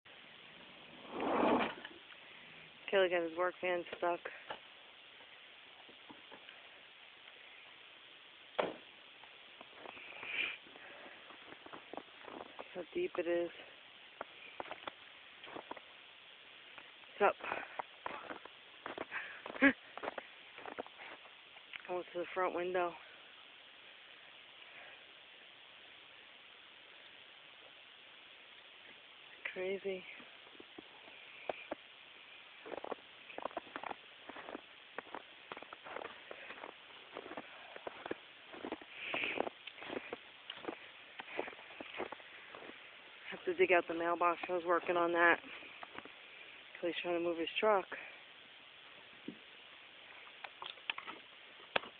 2-10vanstuck.3GP
The 3 feet of show we got and Kelly getting his van stuck